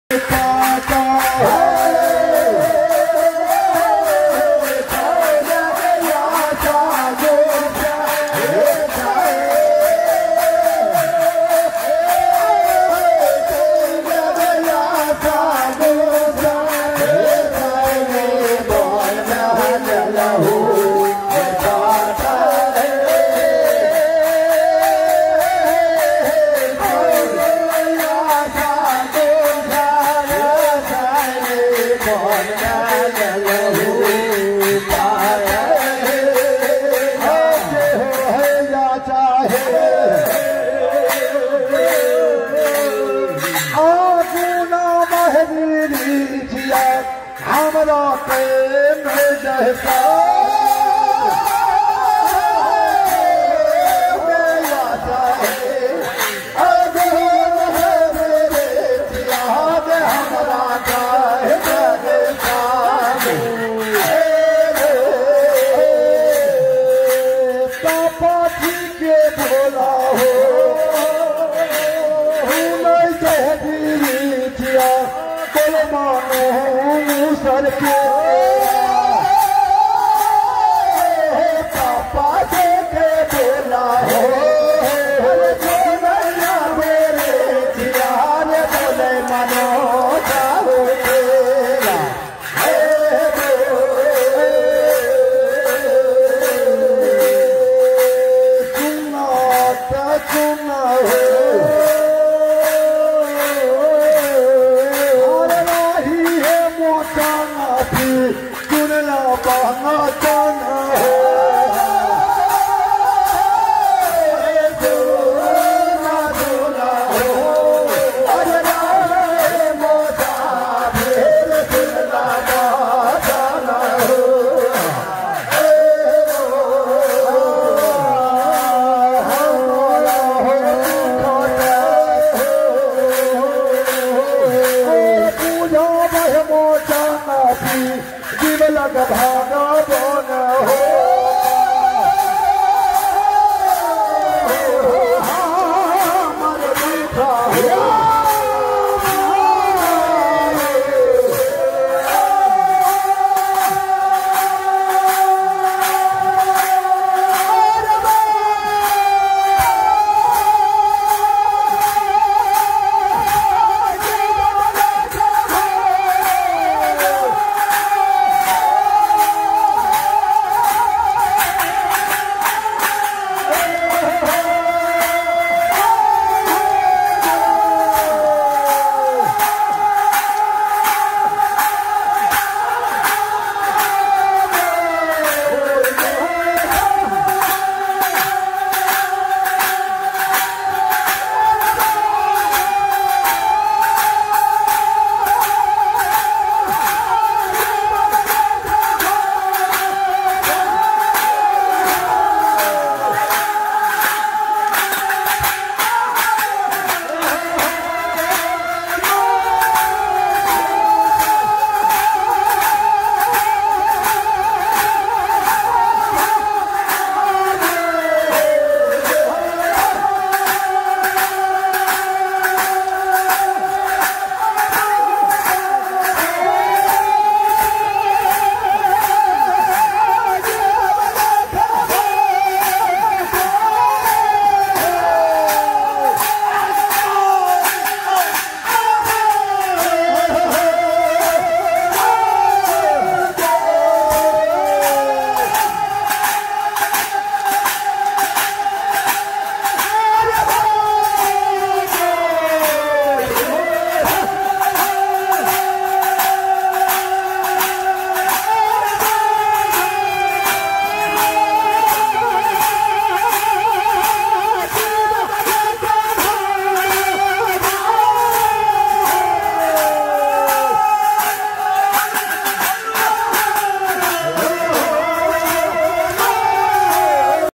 Bhagait - Famous पूजा of Koshi Zone
Bhaigait Puja is being celebrated since ancient time
Now about to extinct
Mainly celebrated in North Bihar,  Koshi zone Madhepura,  Saharasa,  Karihar,  Purnea,  and many more district
In this group one is called BHAGAT who is work as a messenger of GOD
This BHAGAT used to give blessings and also gives some instructions for the progress of their life

Thanks